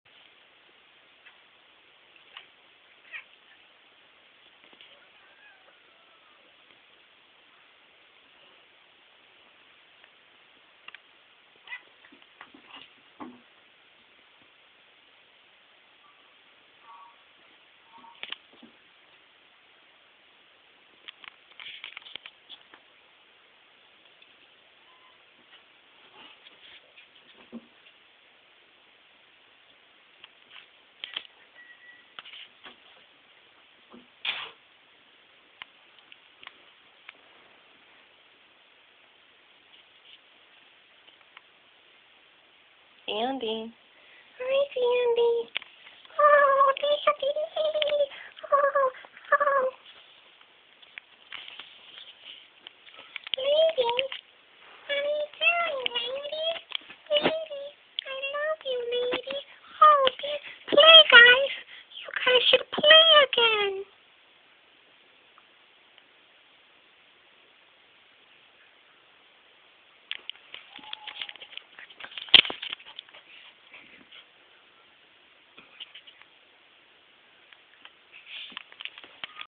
Yorkie and cat play!
This is Dandy my Yorkie playing with Lady my little kitty!